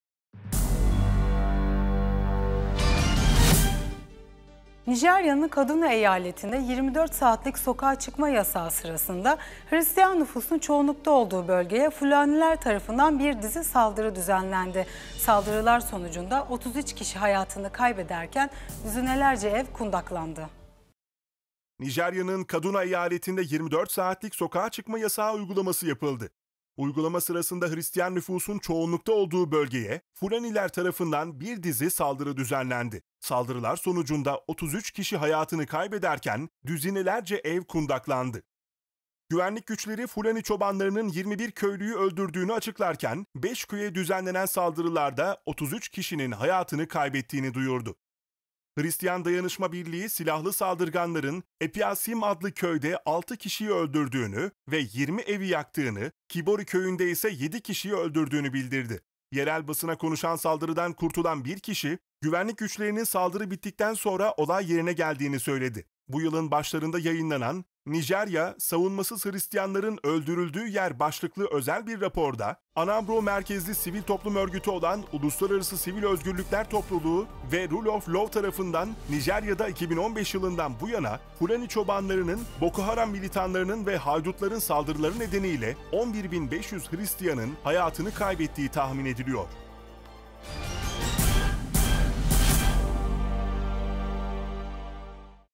Nijerya’da Sokağa Çıkma Yasağı Sırasında Hristiyanlar Hedef Alındı | 19.08.2020
Nijerya’nın Kaduna eyaletinde 24 saatlik sokağa çıkma yasağı uygulaması yapıldı. Uygulama sırasında Hristiyan nüfusun çoğunlukta olduğu bölgeye, Fulaniler tarafından bir dizi saldırı düzenlendi. Saldırılar sonucunda 33 kişi hayatını kaybederken, düzinelerce ev kundaklandı.
Güvenlik güçleri, Fulani çobanlarının 21 köylüyü öldürdüğünü açıklarken, AFP beş köye düzenlenen saldırılarda 33 kişinin hayatını kaybettiğini duyurdu. Hristiyan Dayanışma Birliği (Christian Solidarity Worldwide), Çarşamba ve Perşembe günleri kamyonlarla gelen silahlı saldırganların Apiashyim adlı köyde altı kişiyi öldürdüğünü ve 20 evi yaktığını, Kibori köyünde ise yedi kişiyi öldürdüğünü bildirdi.
Yerel basına konuşan saldırıdan kurtulan bir kişi, güvenlik güçlerinin saldırı bittikten sonra olay yerine geldiğini söyledi.

Fulani Çobanlar Atakmawei topluluğuna da saldırı düzenledi. Saldırı sonucunda 12 kişi hayatını kaybederken, 10 ev kundaklandı. Hristiyan Dayanışma Birliği, Apyiako köyünde üç kişinin ve Magamiya köyünde ise beş kişinin hayatını kaybettiğini duyururken, her iki köyde de kundaklanan evler olduğu açıklandı. Saldırganların zırhlı bir askeri araca benzeyen kamyon ve askeri motosikletlerle Apyiako köy meydanına geldikleri, ancak kimse tarafından engellenmedikleri öğrenildi.
Hristiyan çiftçiler, Nijerya’nın Orta Kuşağı’nda Fulani çobanlarının saldırılarına sıkça maruz kalıyor. Ayrıca ülkenin kuzey bölgelerini tehdit eden Boko Haram terör örgütünün de hedefinde Hristiyan köyler bulunuyor.

Hristiyan Dayanışma Birliği (CSW) Genel Müdürü Mervyn Thomas, ‘‘Bu saldırıların devam etmesi, Vali el-Rufai’nin başarısız olan 24 saatlik sokağa çıkma yasağının sürecini ve kapsamını uzatma kararının bir sonucu. Sokağa çıkma yasağı köylüleri korumak yerine, onları saldırganların hedefi haline getiriyor. Hayatta kalanların güvenlik personellerinin saldırılara müdahale etmemesine ve failleri yakalayamamasına ilişkin sarsıcı iddialarını hızlı ve şeffaf bir şekilde yanıtlaması gerekiyor’’ açıklamasında bulundu.

Bu yılın başlarında yayınlanan ‘Nijerya: Savunmasız Hristiyanların Öldürüldüğü Yer’ başlıklı özel bir raporda, Anambro merkezli sivil toplum örgütü olan Uluslararası Sivil Özgürlükler Topluluğu (International Society for Civil Liberties) ve Rule of Law tarafından, Nijerya’da 2015 yılından bu yana Fulani çobanlarının, Boko Haram militanlarının ve haydutların saldırıları nedeniyle 11.500 Hristiyanın hayatını kaybettiği tahmin ediliyor. (CP)